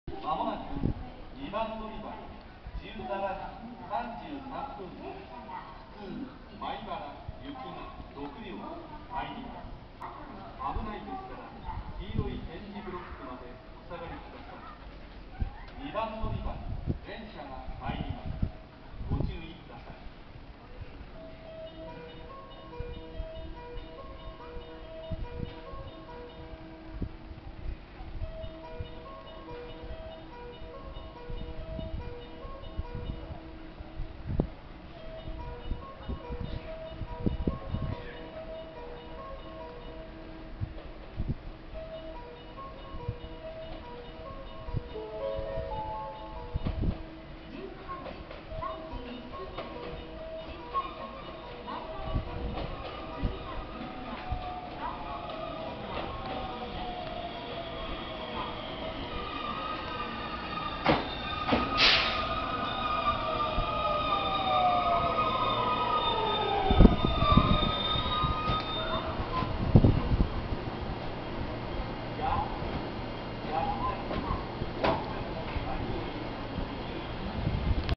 JR琵琶湖線 野洲駅2番ホームに223系普通が到着
2018年5月27日撮影

JR琵琶湖線　野洲駅2番ホームに223系普通が到着します。